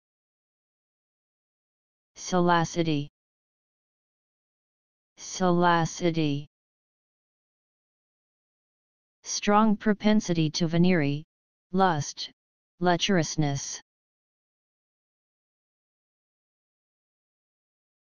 Salacity Meaning
Strong propensity to venery; lust; lecherousness.

In this video, you will learn "Salacity" Meaning and pronunciation in English.

Wordy Affair